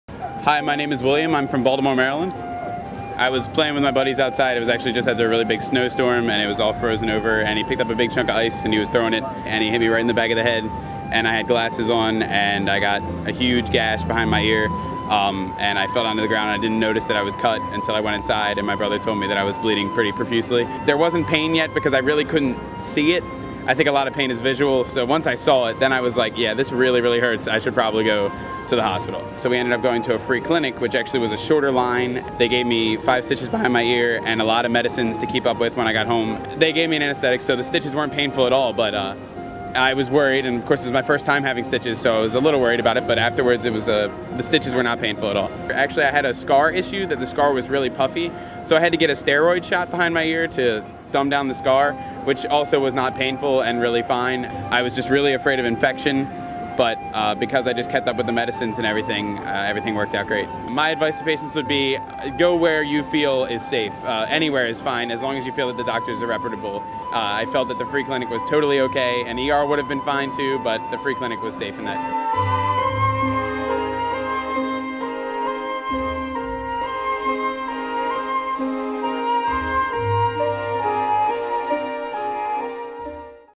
Getting Over the Fear of Stitches: A Winter Accident (Dramatic Health)
"I was just really afraid of infection, but because I kept up with the medicines, everything worked out great." An NYU student relays his story of receiving stitches at a free clinic near his house. The service was excellent and the procedure painless. His advice: if you are worried about scarring go get stitches, they aren't as bad as they seem!